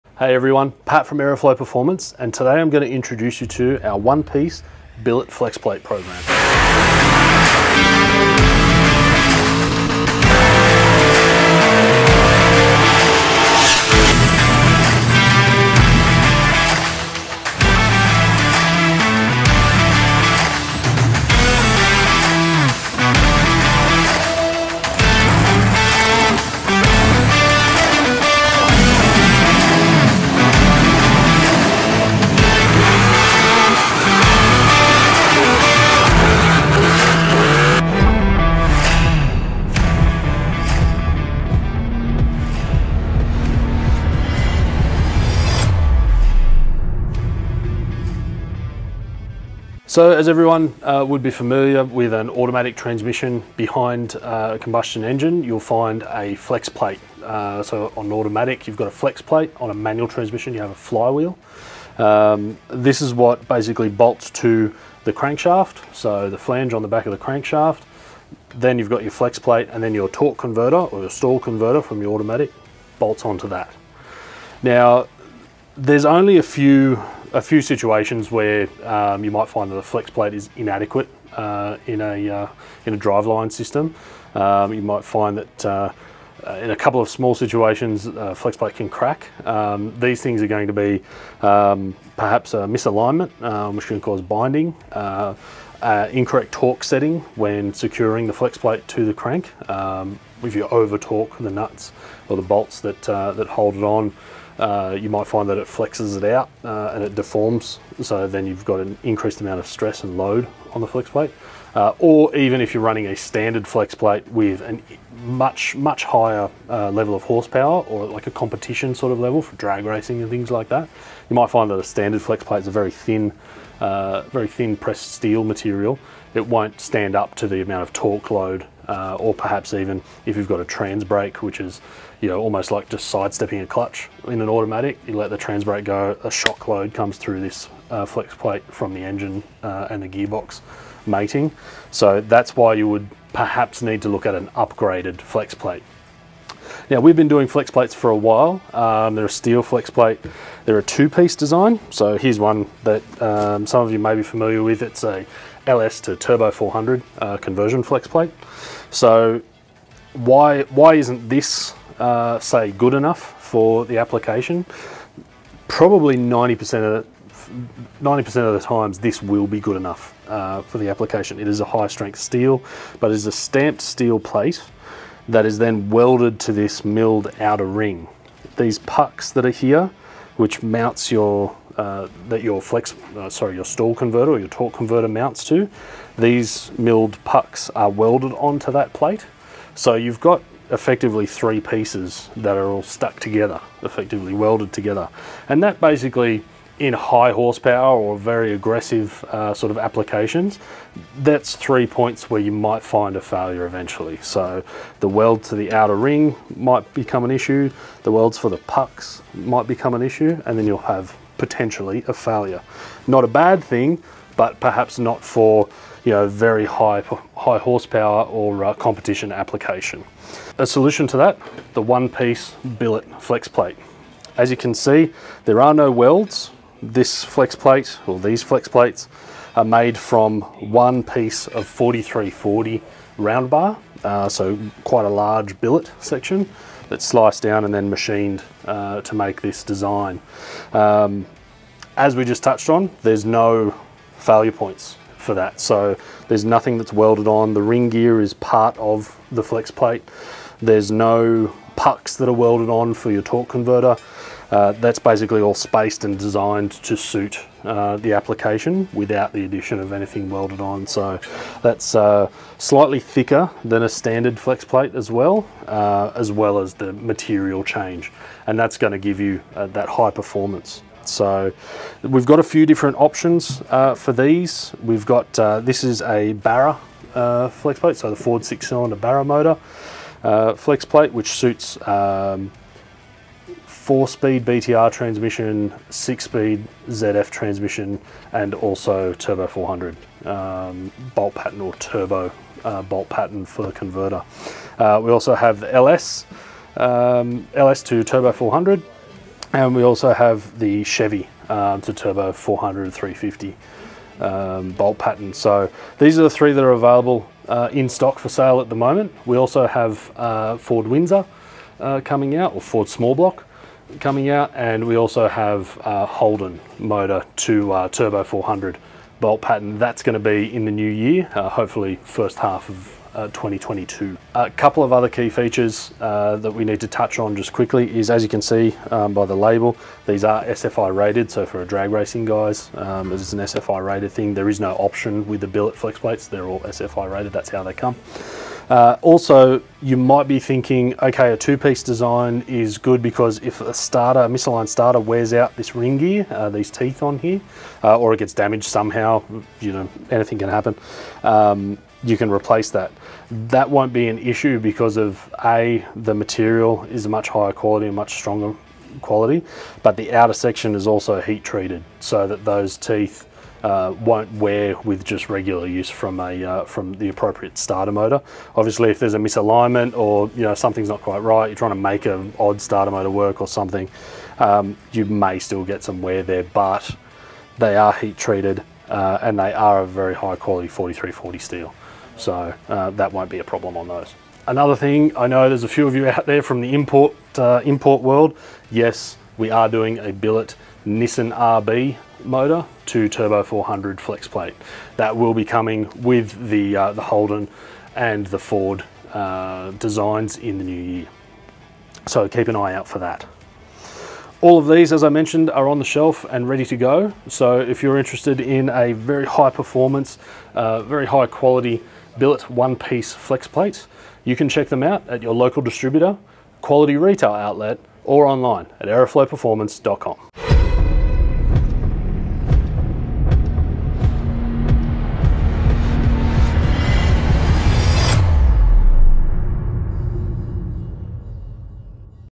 Billet Flexplates
The Aeroflow Performance billet flexplates are the superior choice for quality and precision. The lightweight 1-piece design is stronger and more durable than any 2-piece style of flexplate. Machined from 4340 round bar with precise tolerances. Each flexplate features chamfered gear teeth for better engagement with the least amount of runout to provide the best longevity on the market. All of our billet flexplates are certified to SFI 29.2 and available for many different models. CNC machined from 4340 round bar and heat treated to HRC45-55.